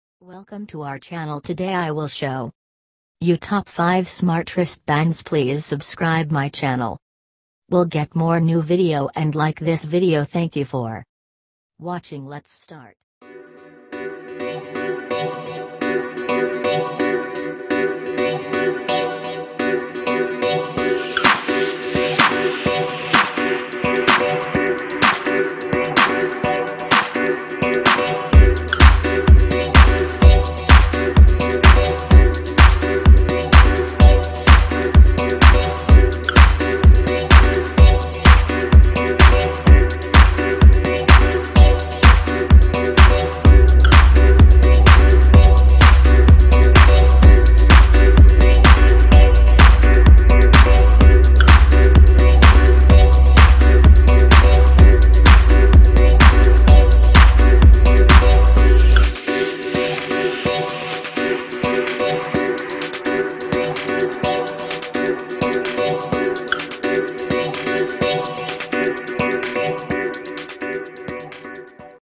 Top 5 bluetooth smart watch android manual 2018 Under 55$
Today i will show You-  Top 5 bluetooth smart watch android manual 2018 Under 55$

1-- Smart Bracelet Activity Fitness Tracker Electronics Wristband

5--Blood Pressure Smart Band Heart Rate Monitor PPG ECG

MORVIDEO

Description:
DYNAMIC HEART RATE and AUTO SLEEP MONITOR : fitness tracker monitors dynamic heart rate , uploading the real time data of heartbeats to phone app . Also support automatic sleep monitoring , it records deep , light , awake time and whole sleep time . All health data showas a chart , clearly acknowledge your physical fitness status at whole day .

Main Functions: count-down, Step count,24hour continuous blood pressure monitoring, 24 hour continuous heart rate monitoring, Stopwatch, Reminder, Running mode, Multi-display mode, Fatigue test, Calorie, mileage, Call reminder, Refuse call, Wechat movement,Notification, Sleep Monitoring, Photograph, Waterproof, Phone tracking, Heartrate warning, Sedentary Reminder.